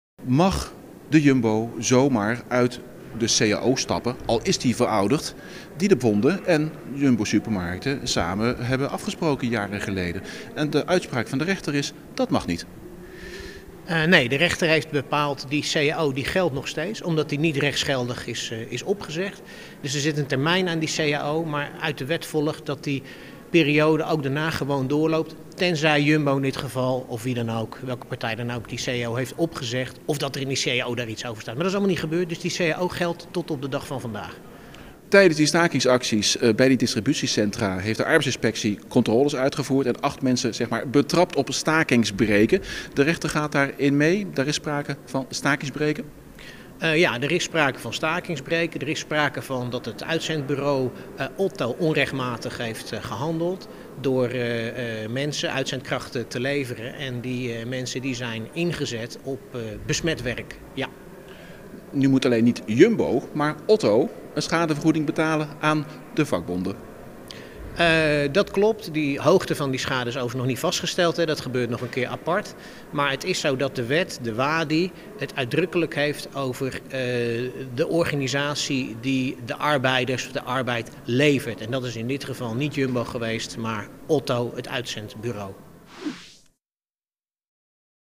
Vakbonden krijgen gelijk in ruzie met Jumbo
Supermarktketen Jumbo uit Veghel moet zich aan de oude cao voor de distributiecentra houden, ook als is die verlopen. Dat bepaalde de rechtbank in Den Bosch donderdag in een rechtszaak die door vakbonden FNV en CNV was aangespannen.